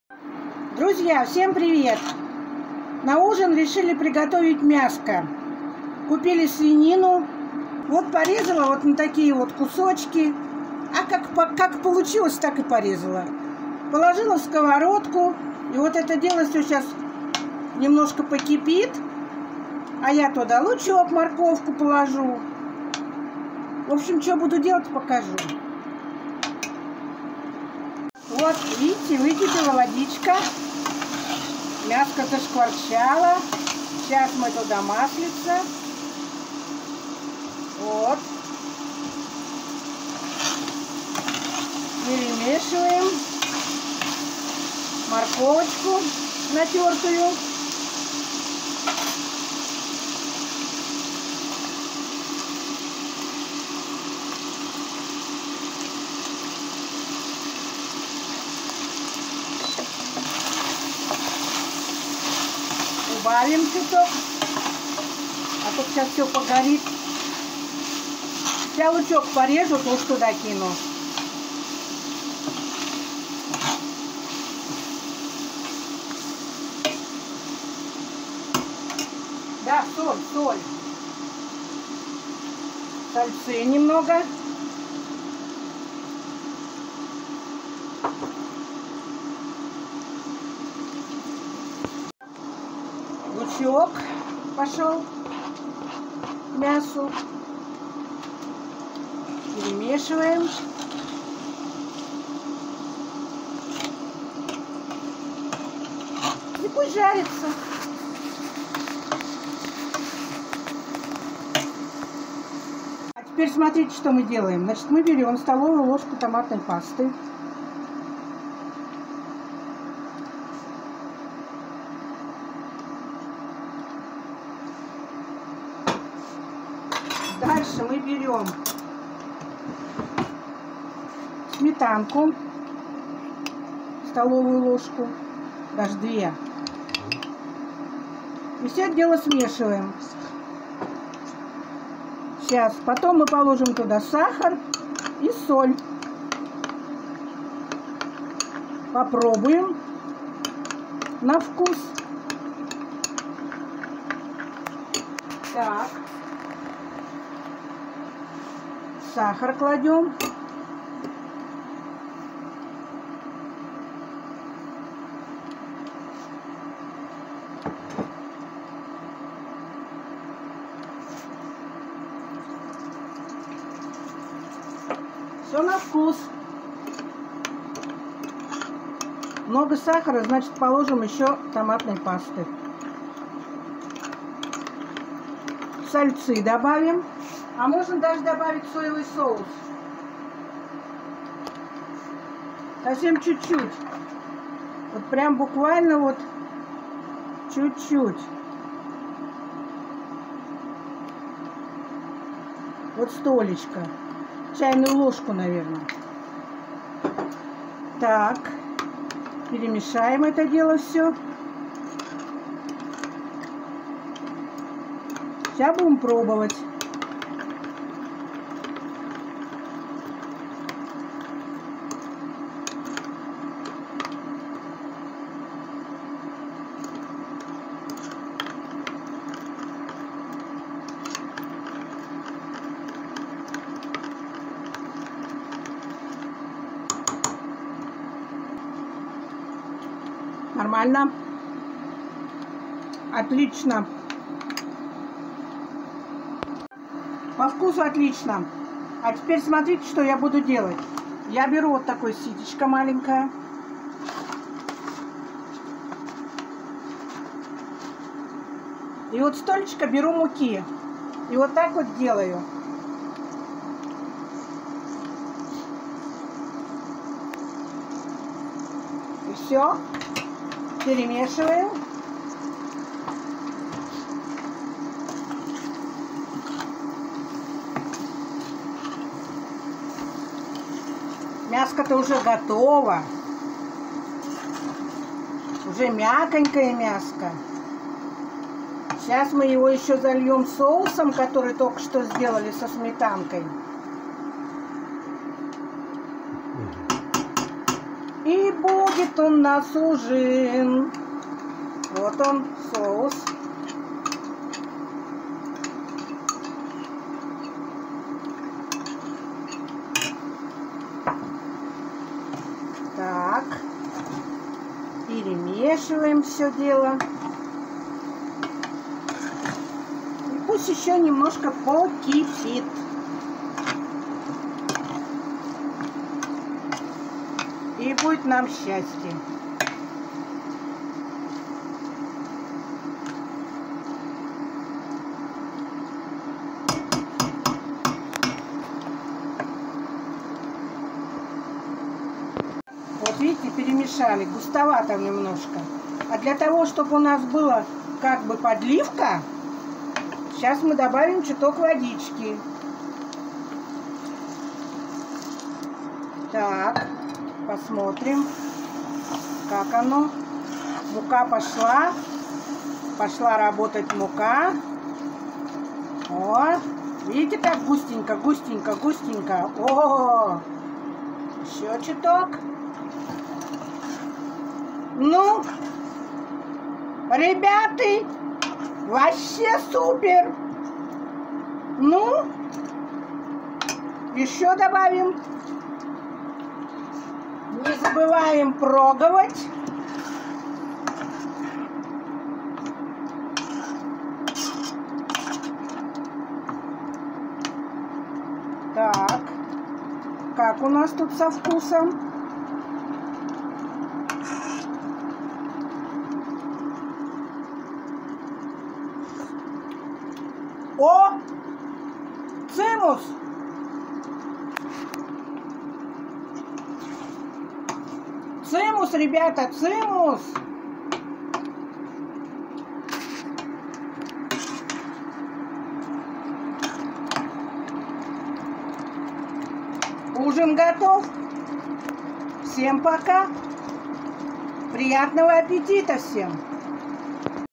вкуснейшее мясо по деревенски на ужин delicious rustic meat for dinner
Приготовили свинину. Соус получился очень вкусный, сколько граммов во всех ингредиентах, а всё по вкусу и и на "глаз".
Pork was cooked. The sauce turned out to be very tasty, how many grams are in all the ingredients, and everything is to taste and to the "eye". Enjoy your viewing.